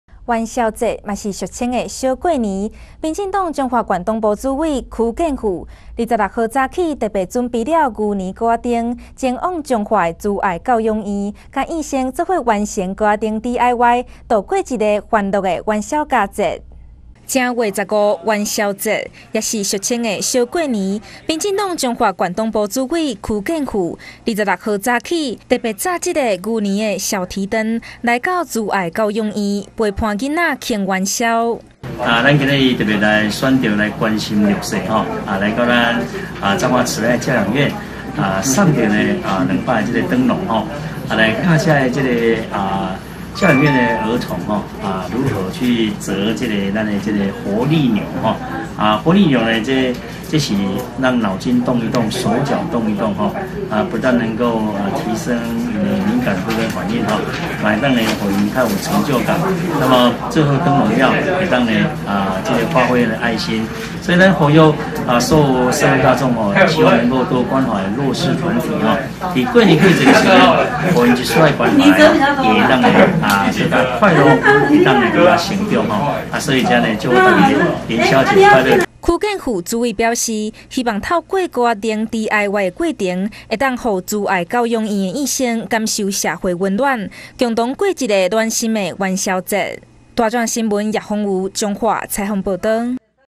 1100226邱建富攜小提燈 伴慈愛教養院生慶元宵
元宵節又是俗稱的小過年，民進黨彰化縣黨部主委邱建富，26日上午，特別準備牛年小提燈，前往彰化慈愛教養院，與院生一起DIY完成小提燈，共度元宵佳節。